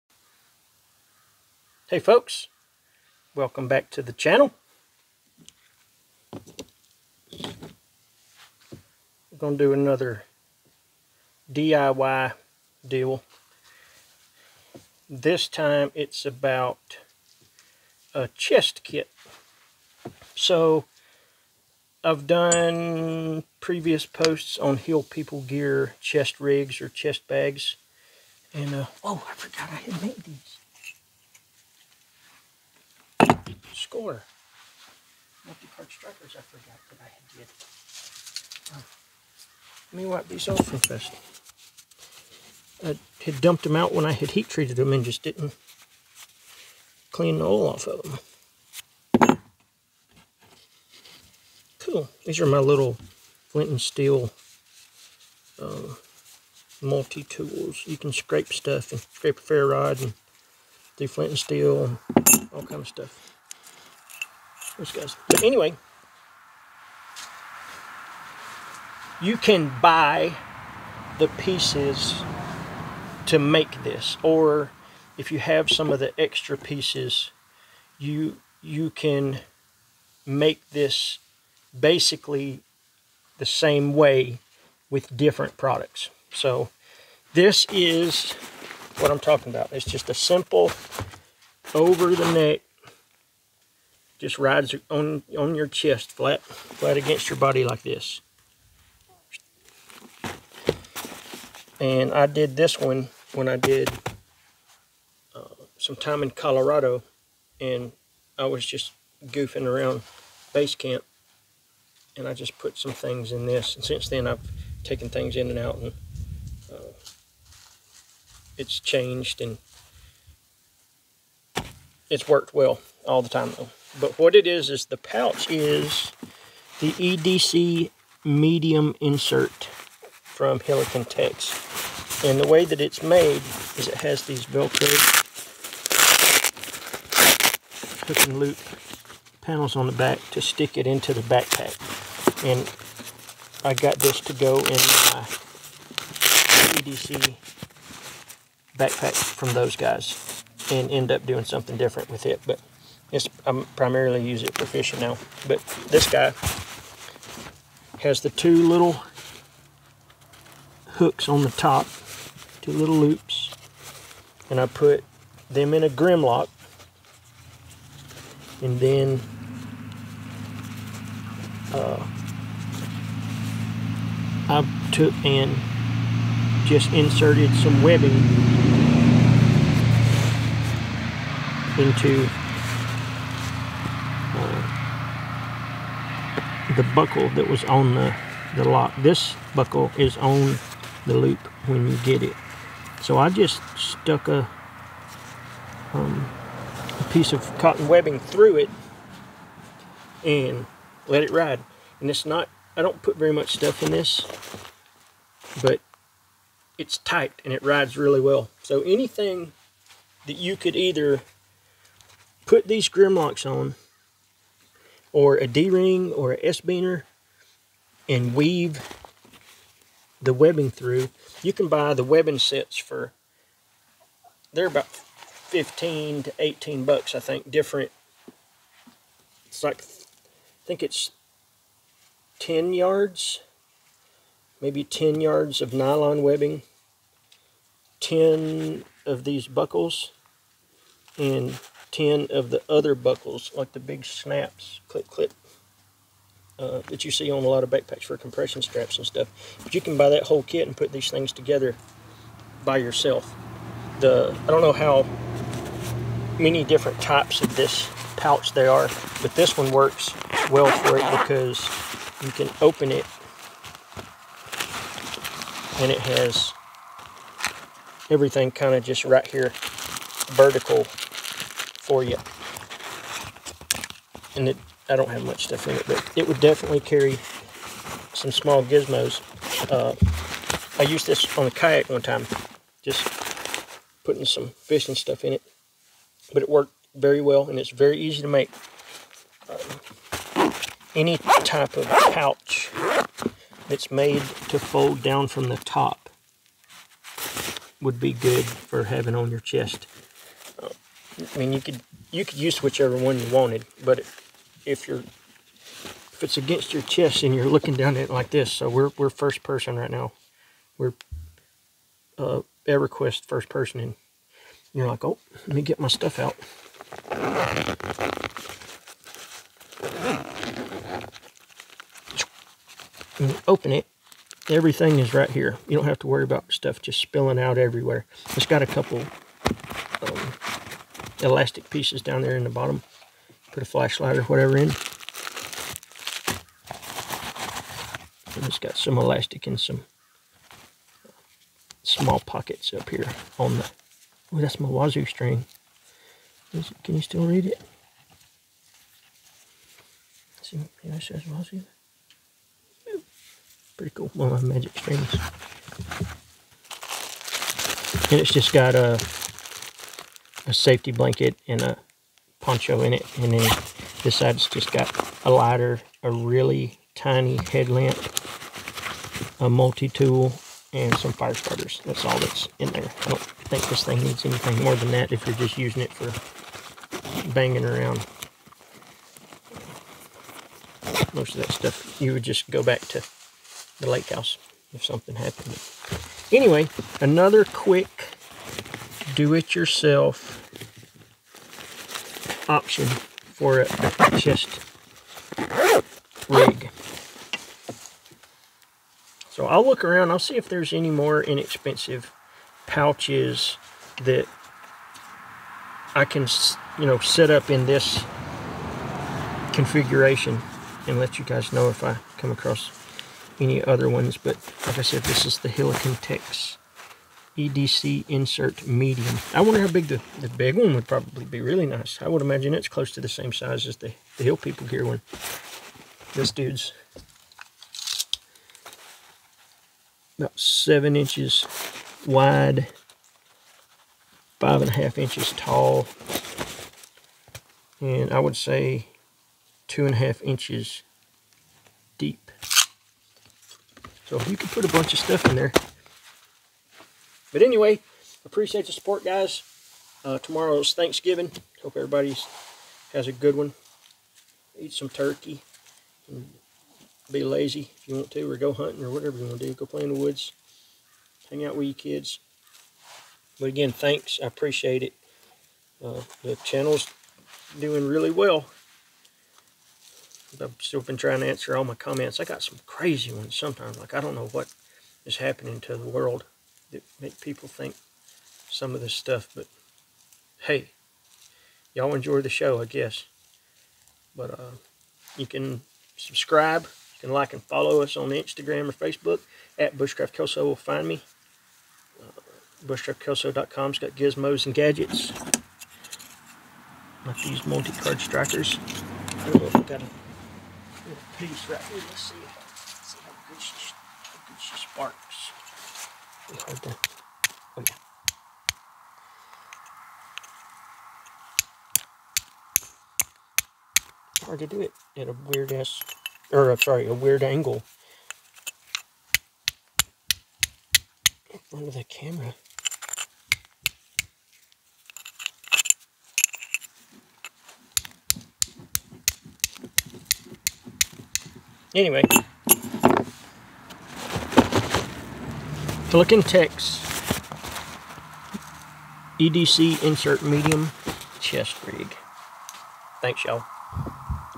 Easy chest rig build with with minimal "crafting"
Great option for a really small kit option.
@HelikonTex EDC medium insert
Approx 3 feet of webbing
2 buckles (on the pouch ) but will need 2 on a different pouch.

Bust attach the webbing and your ready
Really easy and works great for most applications it would hop around if you were running or jumping but just walking or in a Canoe it would be nice